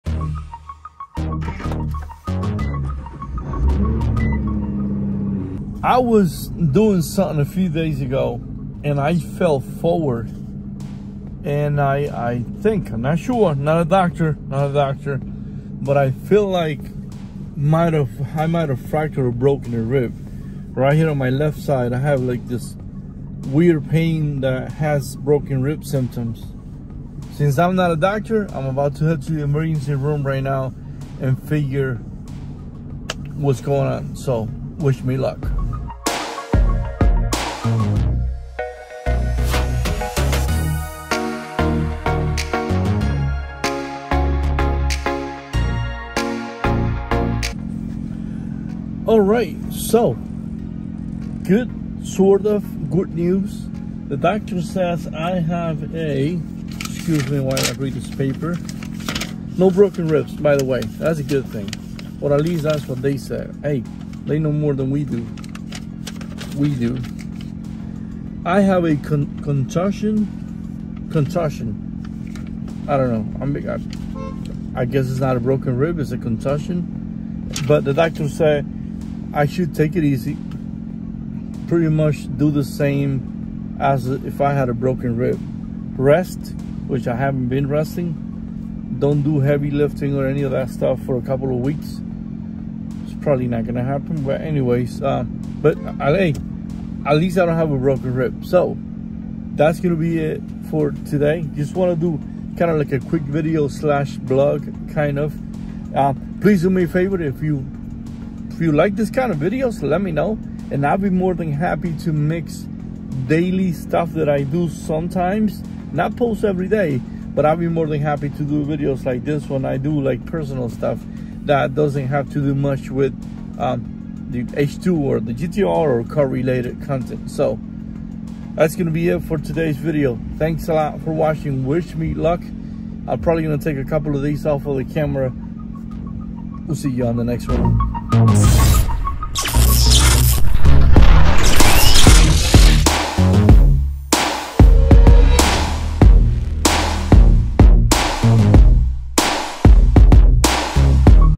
I ALMOST BROKE MY RIBS
Thanks again for watching luckly my ribs was not broken but it sure hurts alottt . thanks again for watching .